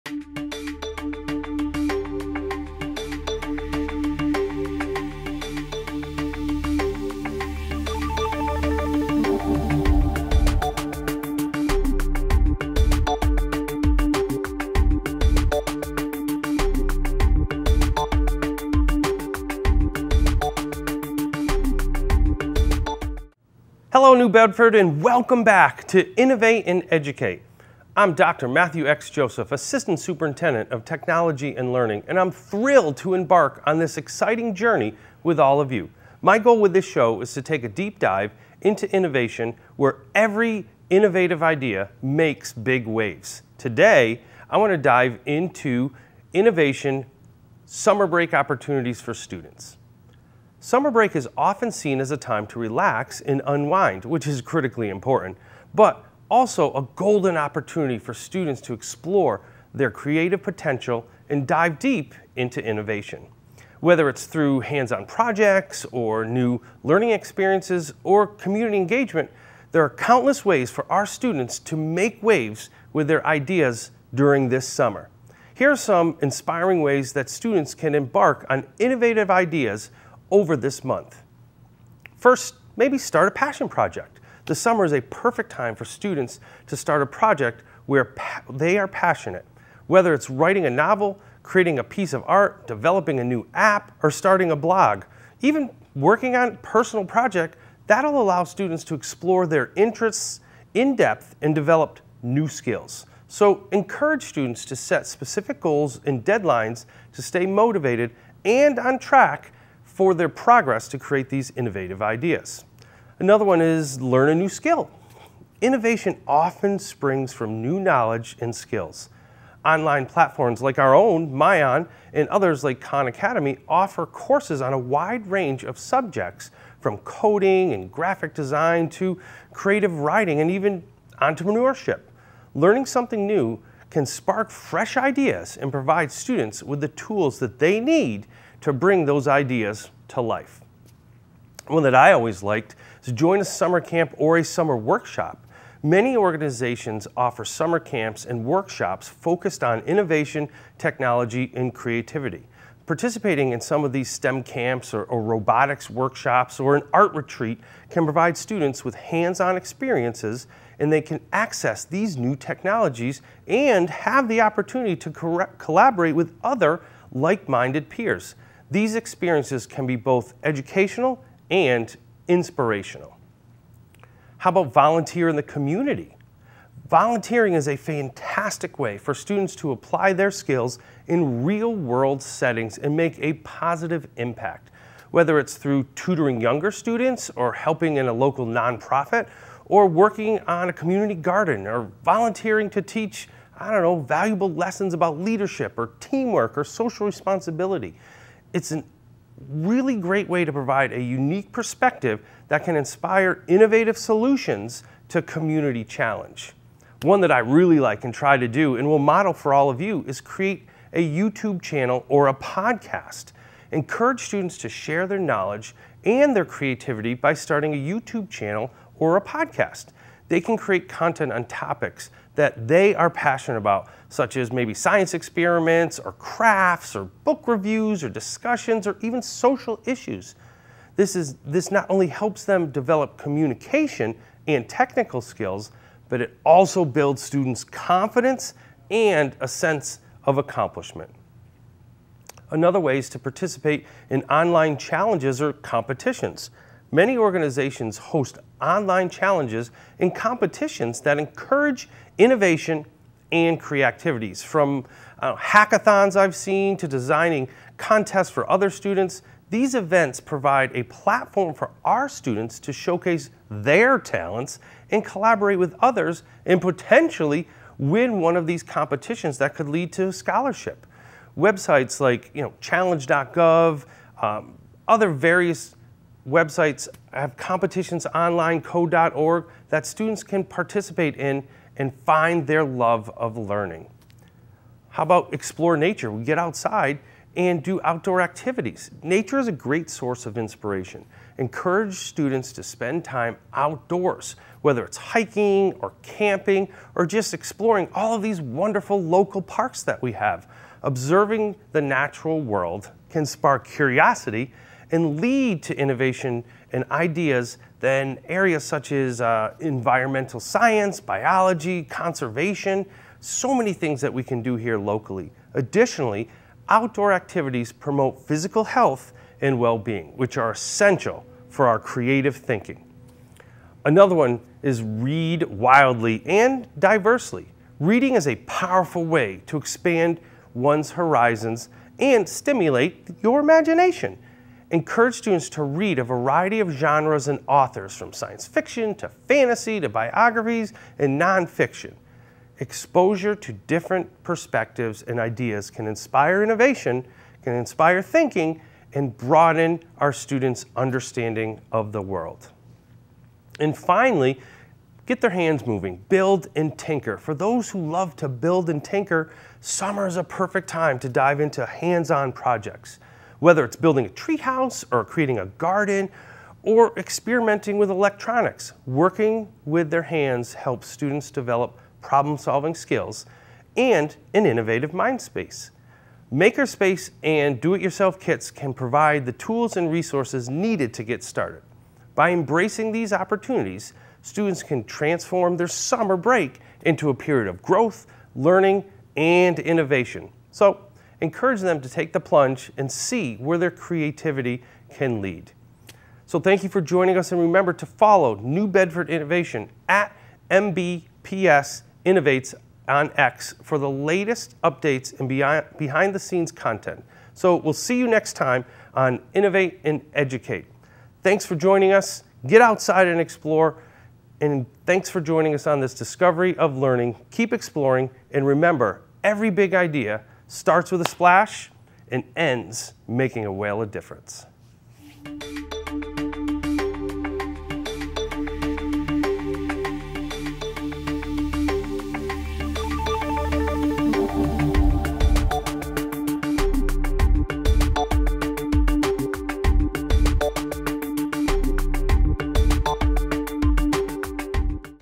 Innovate and Educate Ep2
In this episode of "Innovate and Educate," host Dr. Matthew X. Joseph, Assistant Superintendent of Technology and Learning for New Bedford Public Schools, talks about how summer break is a perfect time for students to explore innovative opportunities.  August 12, 2024.